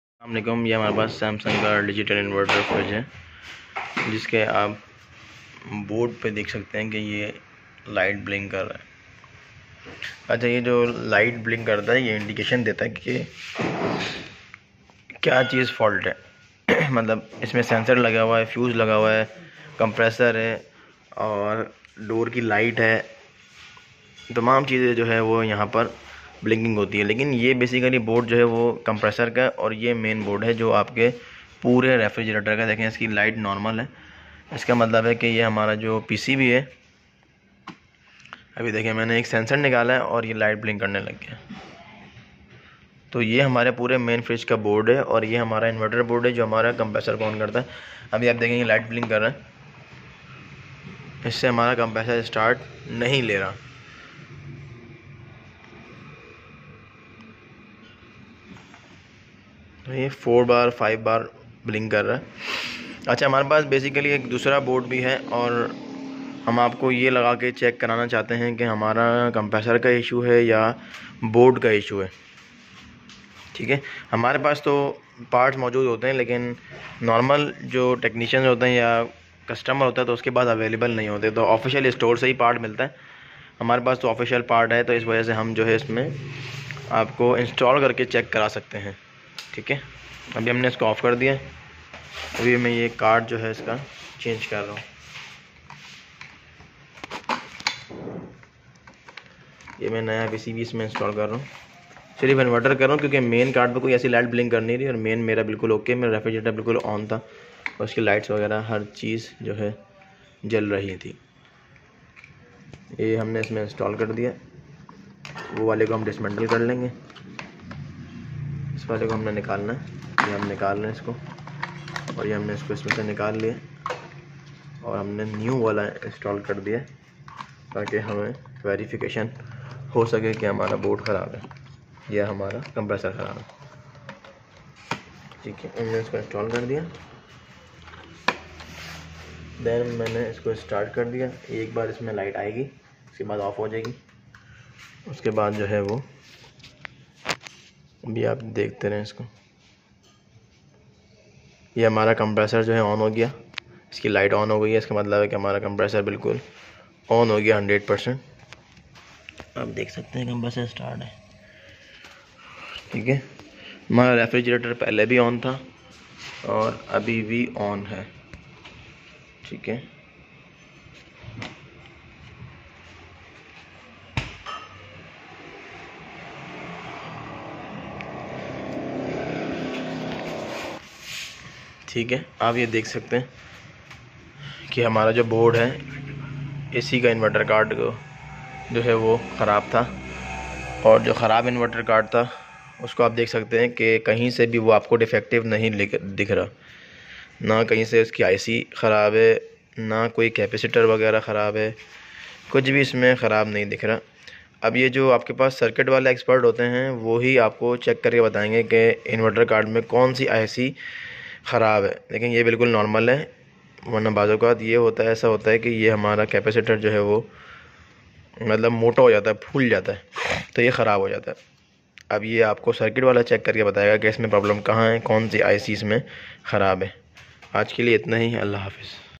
How to diagnose inverter card error code by red light blinking on Samsung Refrigerator
In this video, I wil show you How to diagnose inverter card error code by red light blinking on Samsung Refrigerator

My AC Support shows you how to "Fix it Yourself and Do it Right the First Time"